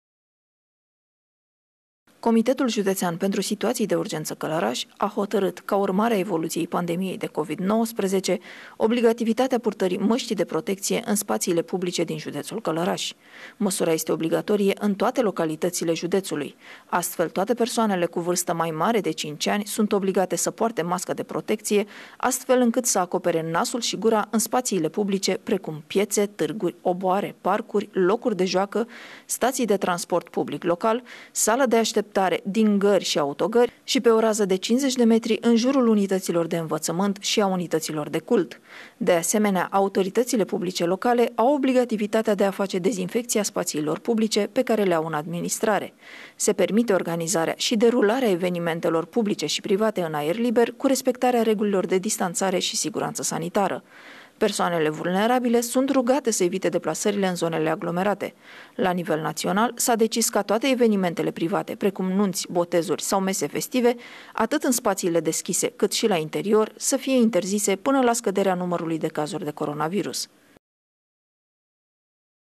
Masca obligatorie în spațiile publice
Se permite organizarea și derularea evenimentelor publice și private, în aer liber cu respectarea regulilor de distanțare și siguranță sanitară.